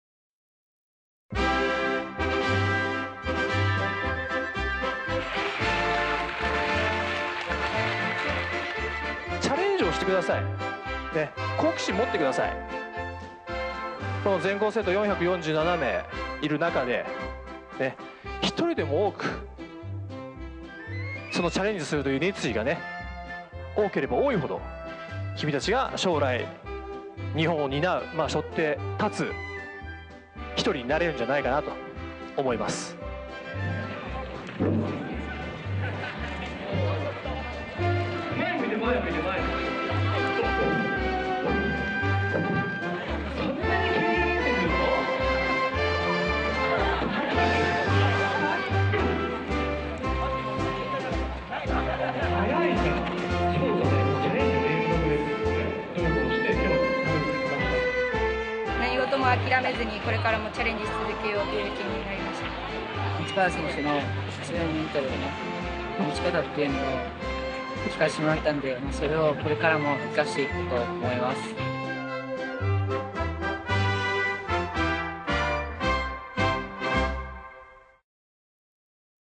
夢・未来プロジェクト 塚原直貴選手 来校
北京オリンピック銀メダリスト塚原直貴選手が区内の中学校を訪れ、講演会や実技指導を行いました。オリンピアンの実技指導に目を輝かせる子どもたちの様子をご覧ください。